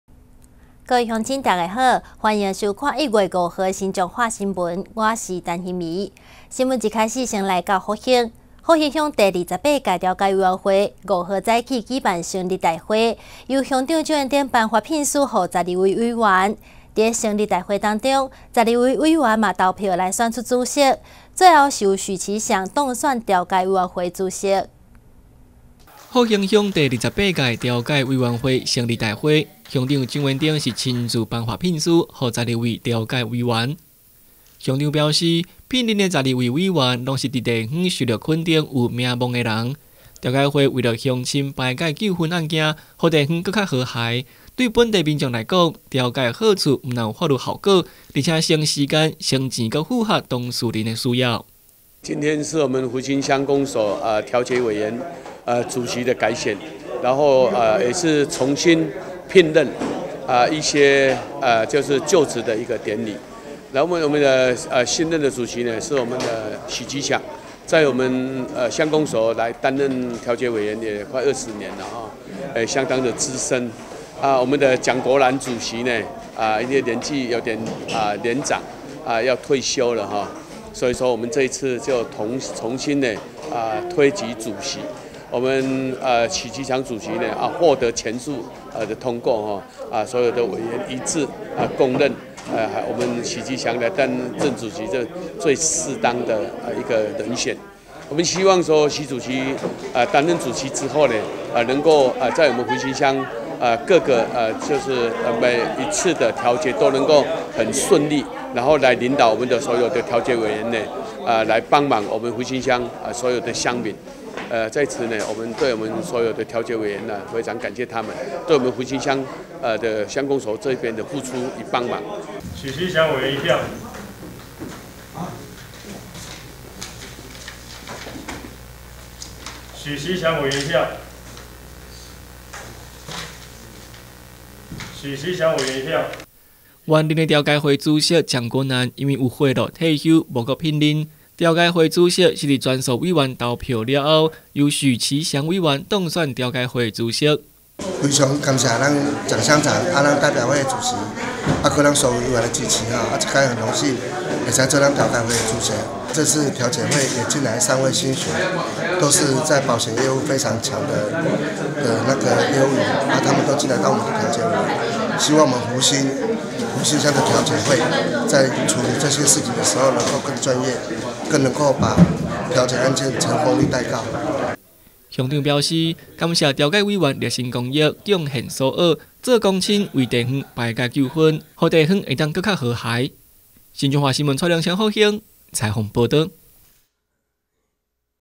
新彰化新聞20230105 福興鄉第28屆調解委員會 許琦祥當選調解會主席
福興鄉第28屆調解委員會，5日上午舉行成立大會，由鄉長蔣煙燈頒發聘書給12位委員，成立大會中，12位委員投票選出主席，最後在全數委員都支持之下，由許琦祥當選調解會主席。